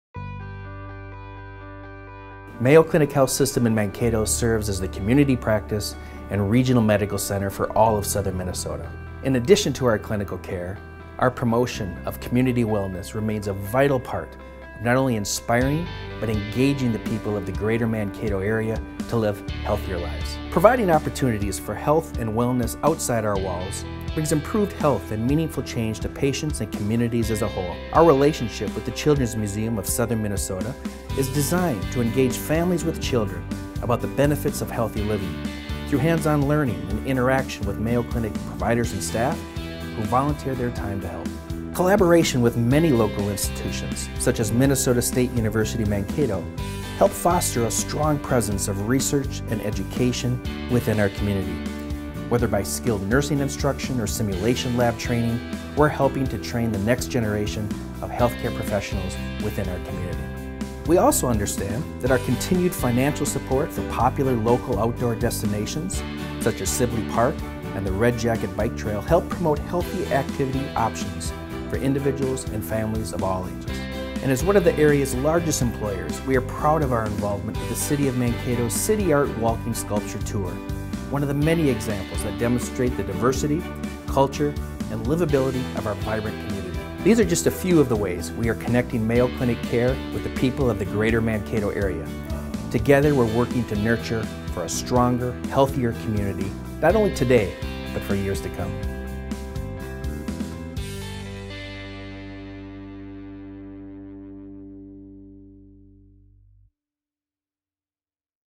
Mayo Clinic Health System in Mankato - Community Wellness
Mayo Clinic Health System has a role in promoting community wellness and remains a vital part of helping inspire and engage the people of Mankato and surrounding area to live healthier lives. Together, we’re working to nurture a stronger and healthier community today — and for years to come.